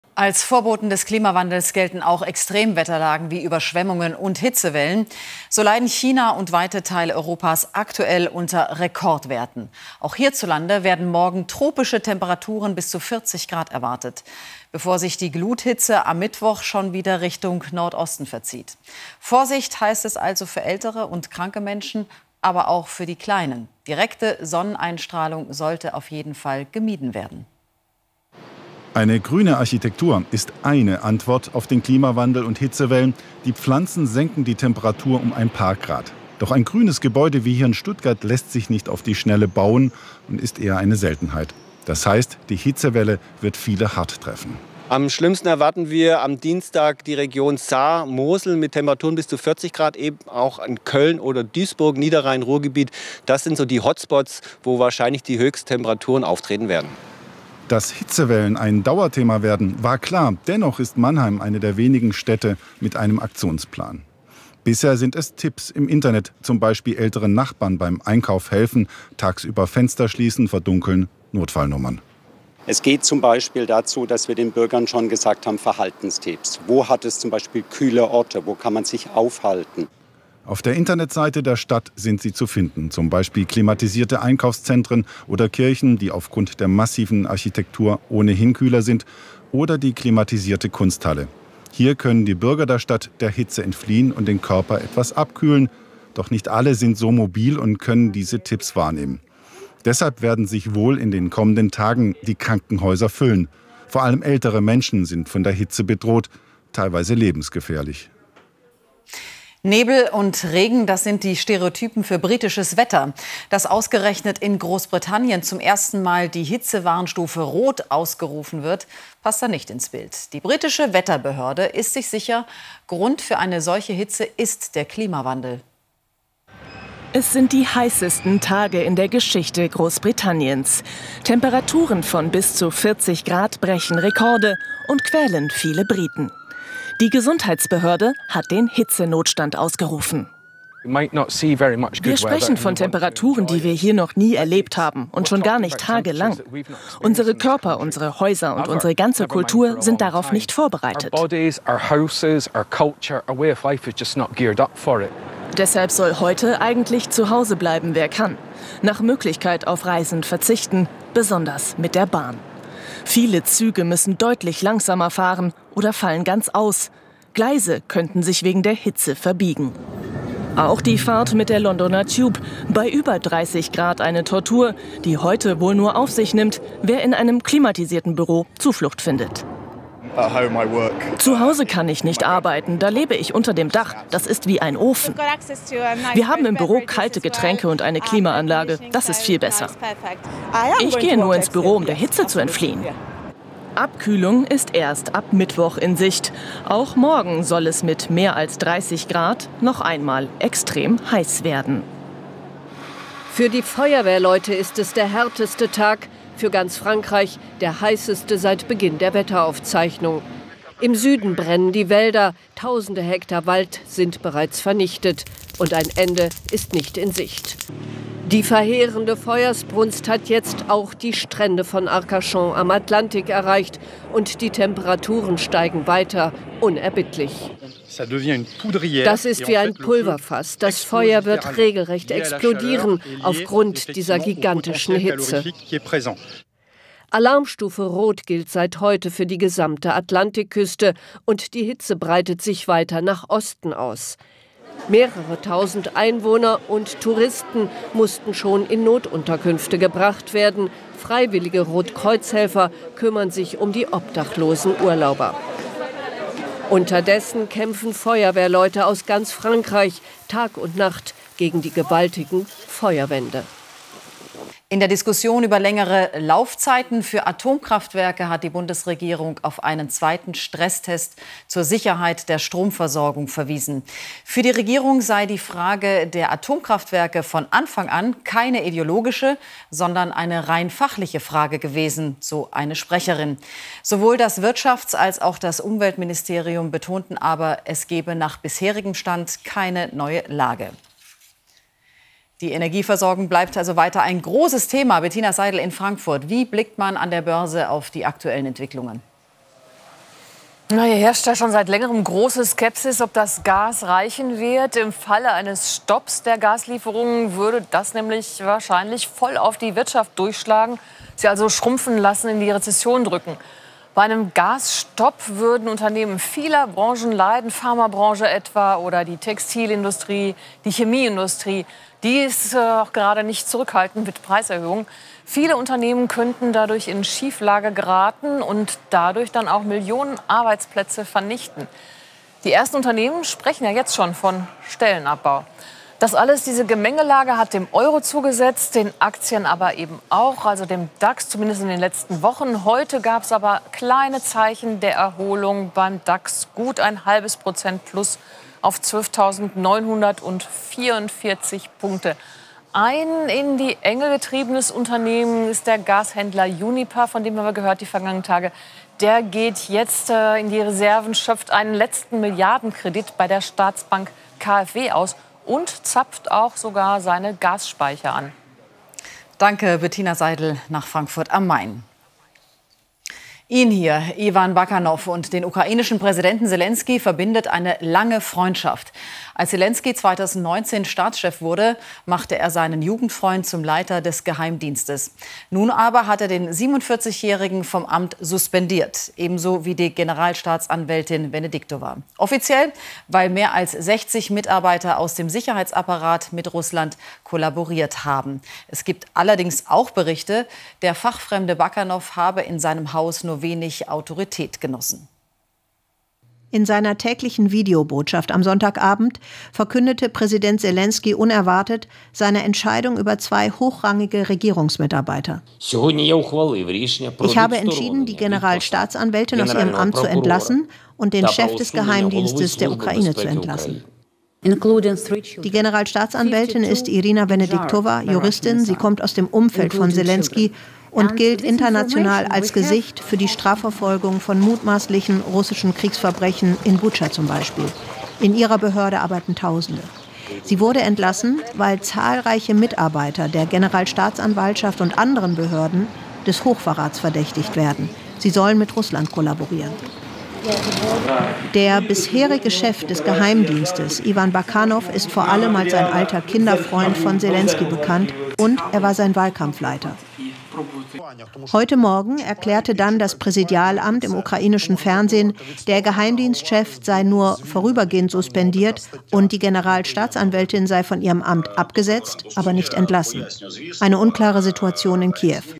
Hitze in EU, Stresstest für AKW in De., Die Börse, Leitende Verwaltungsbeamte in Ukraine suspendiert
Die Börse, Ukrainischer Präsident Selenskyj suspendiert Geheimdienstchef und eine Generalstaatsanwältin,